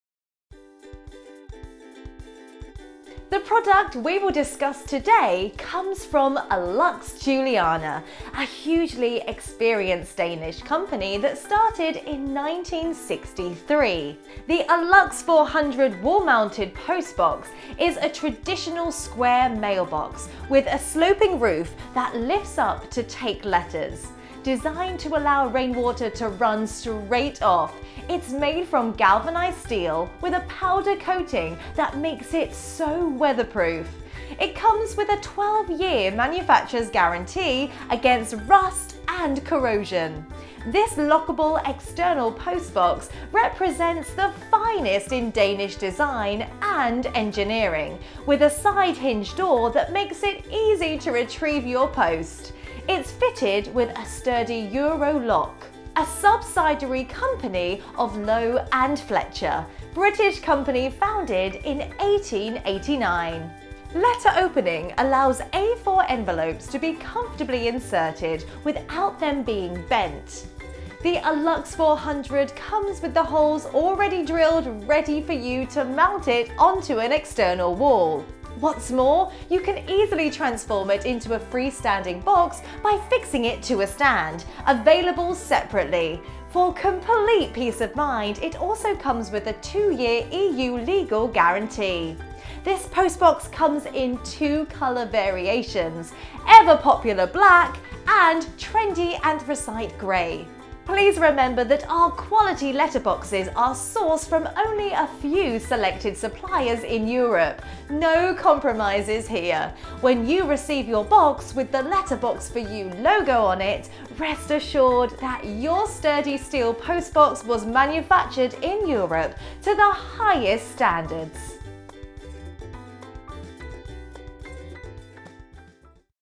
Wall mounted post box Allux 400 from Letterbox4you- overview
Allux 400 wall mounted post box is only one of many you will find in our wall mounted range.